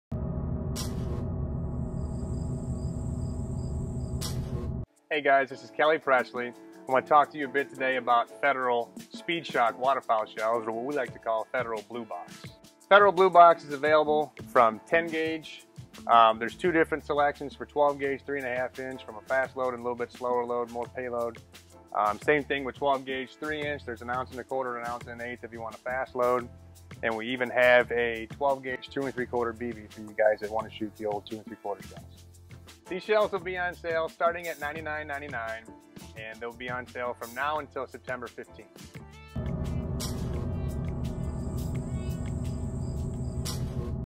Federal Speed Shok (Blue Box) at Presleys Outdoors
Kelly Presley talks about a great deal on Federal Blue box steel waterfowl shotgun shells.